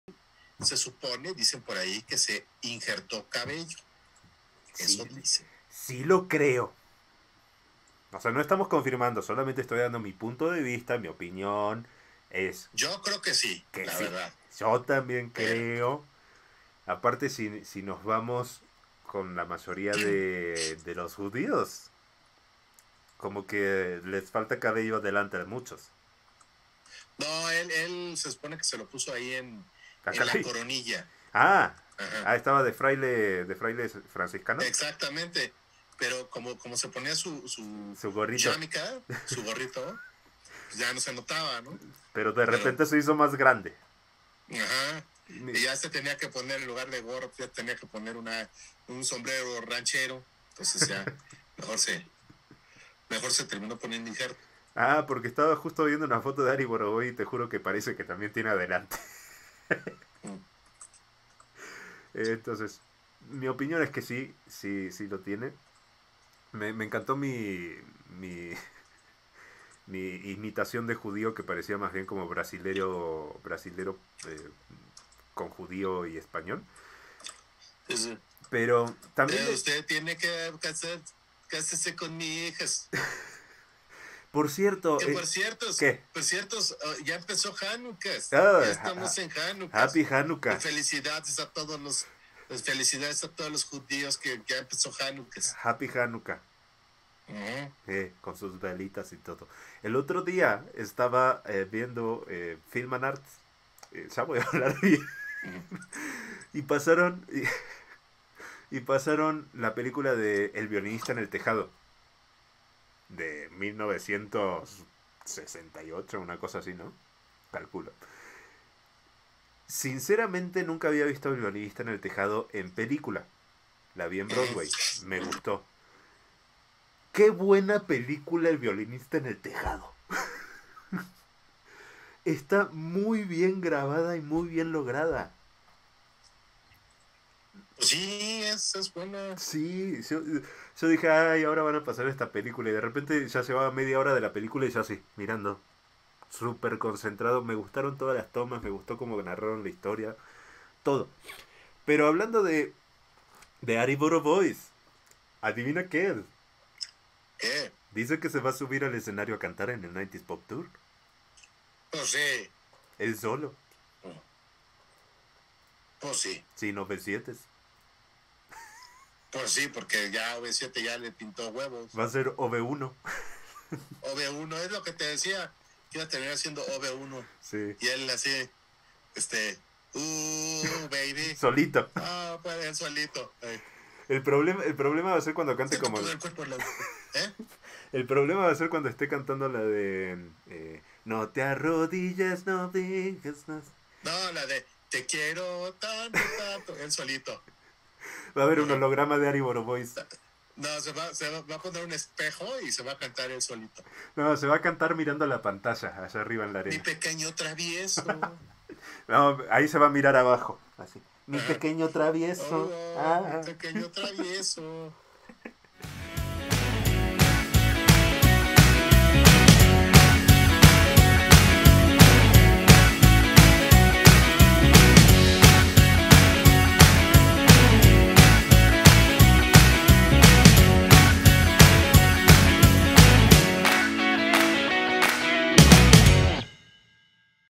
ARI BOROVOY VUELVE SOLO AL 90s POP TOUR
Ari dijo que si se sube al escenario en el 90s pop tour... Pero ¿Solo?